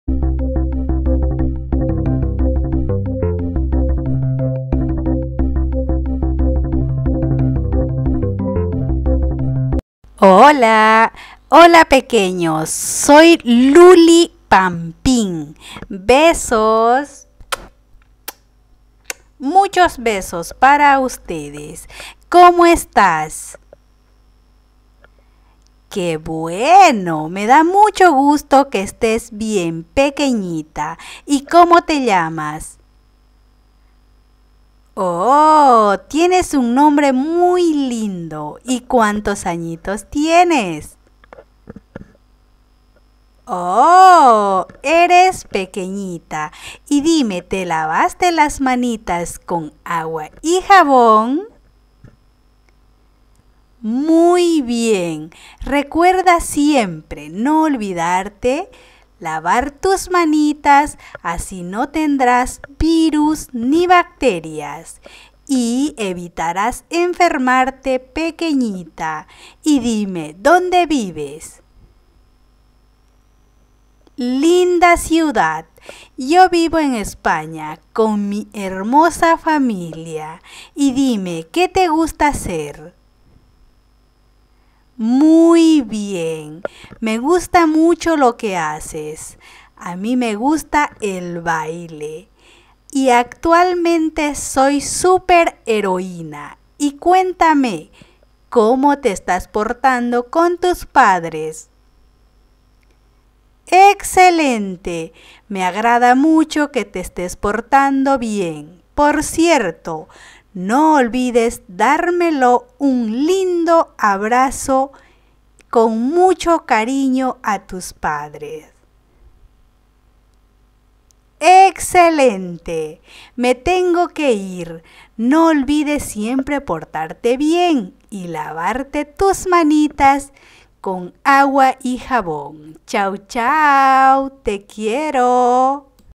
Llamada Falsa de Luli Pampin
llamadastelefonicas Luli Pampin#call

Luli Pampin quiere recomendar que no se olviden de lavarse las manitas
tus hermosos y bellos hijos para que siempre estén super limpios

Llamada de Luli Pampin
Llamada a Luli Pampin
Llamada Luli Pampin